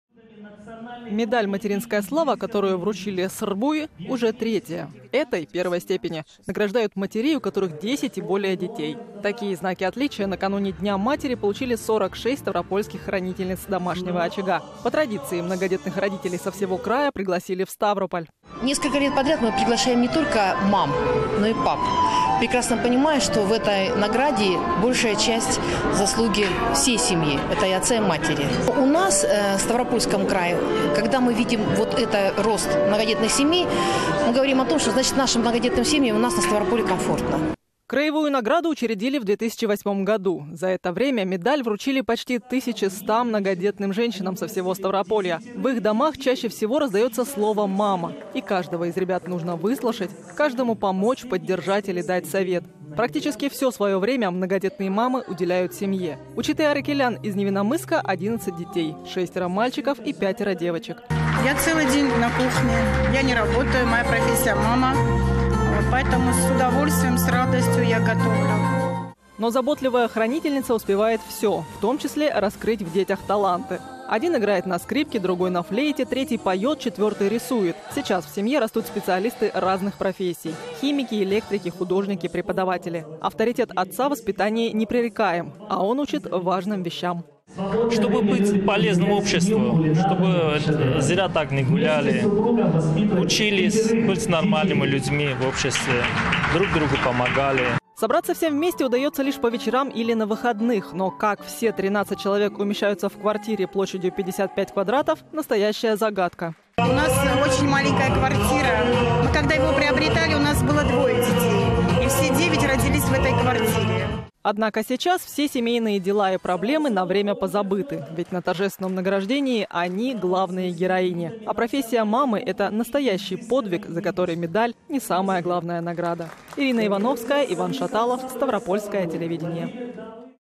В Ставрополье многодетным женщинам вручили медали "Материнская слава"
На Ставрополье наградили не только воительницу из магазина, но и хранительниц очага. В преддверии Дня матери многодетным женщинам региона вручили медали "Материнская слава". Наш корреспондент пообщалась с одной из хранительниц большого семейного очага.